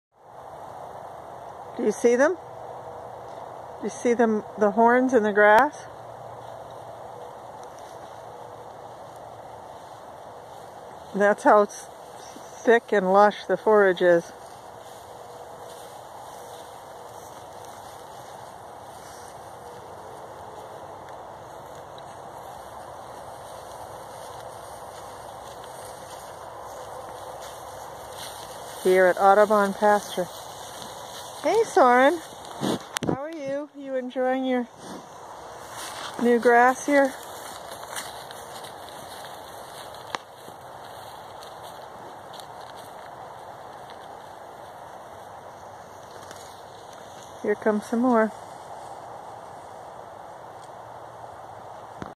Audubon Pasture - Oct 2018
Whoever said grazing is done on Oct 20th has clearly never stock-piled forage. This forage at the Audubon pasture (owned by St. Paul's School) will keep our herd of 40 cattle fed well into December!